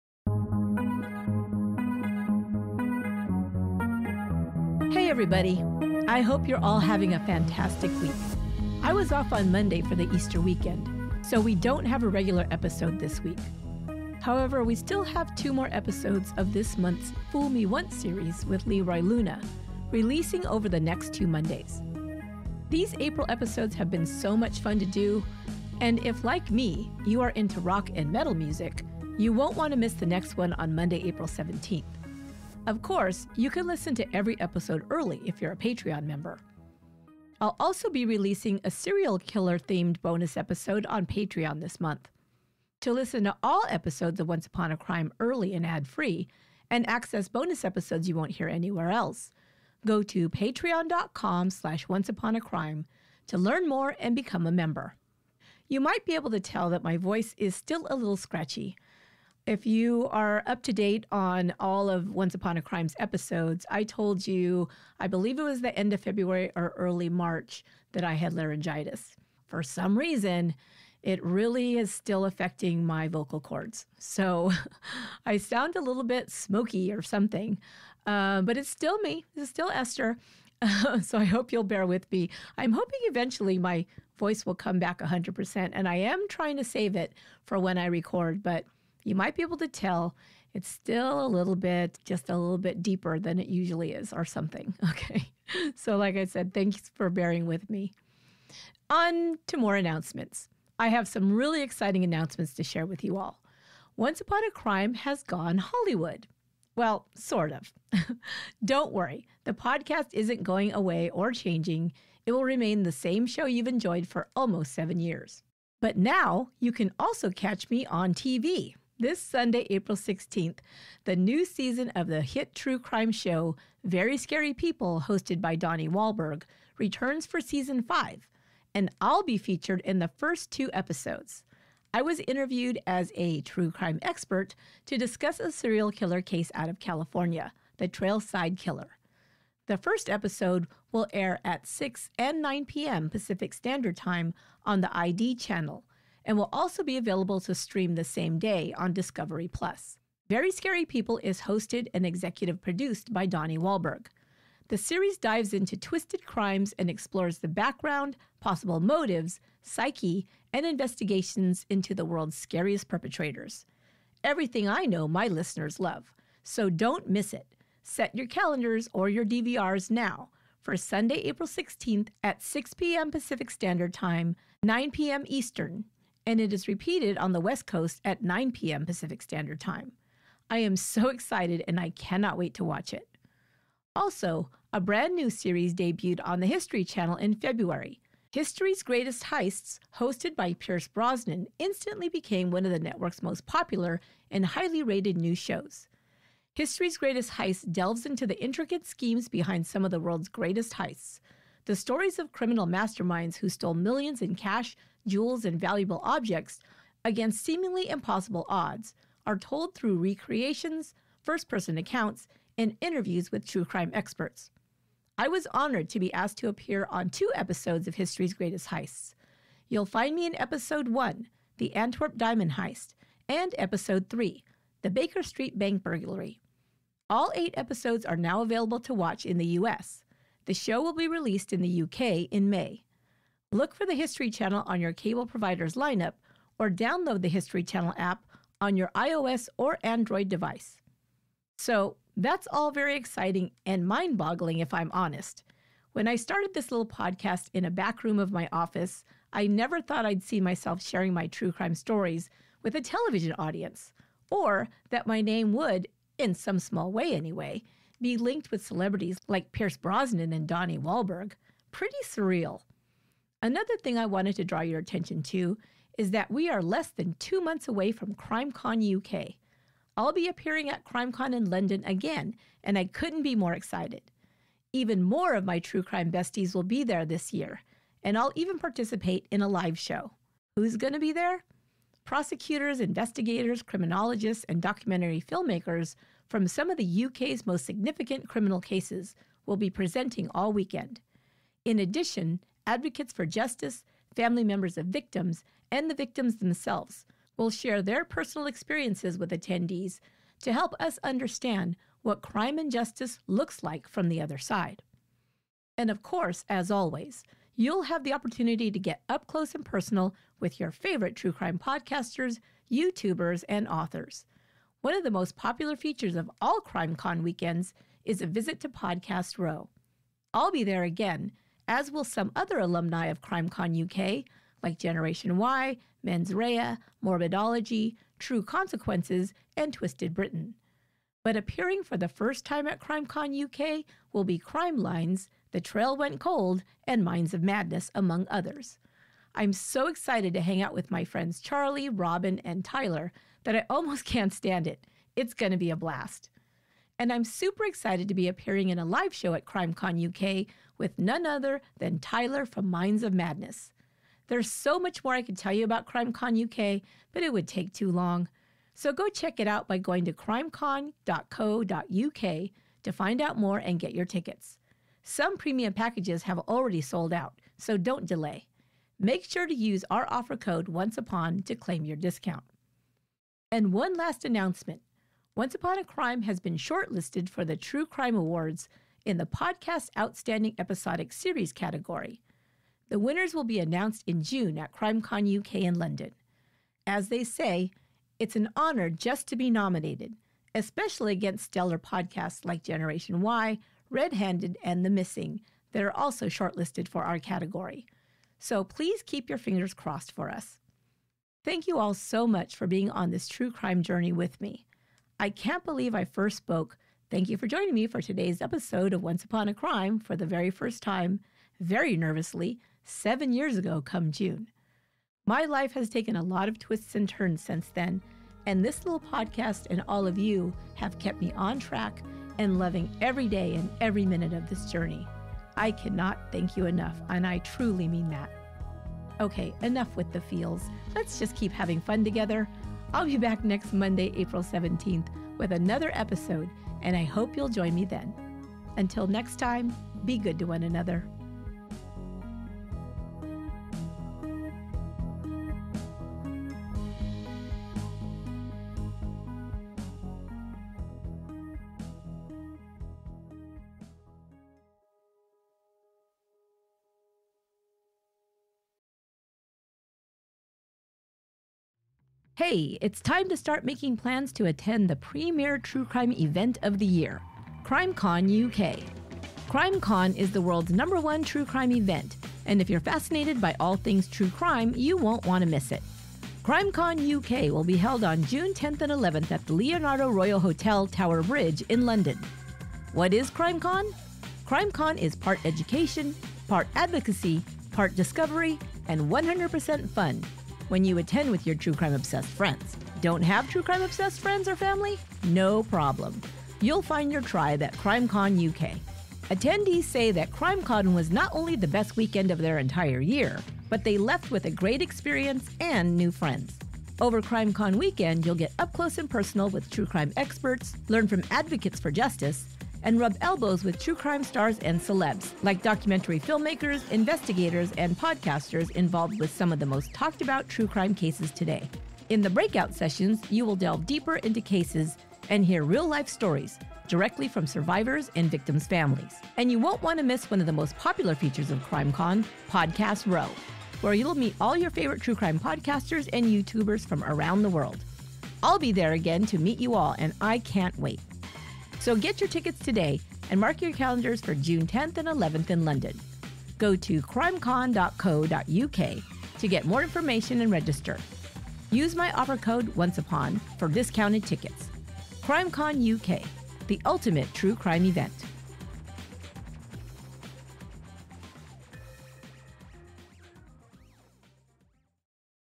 Once Upon Has Gone “Hollywood”! - Updates and Announcements
We have some exciting announcements to share with you in this short update episode.  Find out where and when you can see Esther appear on television and in person!

We love you all, and thank you so much for your support!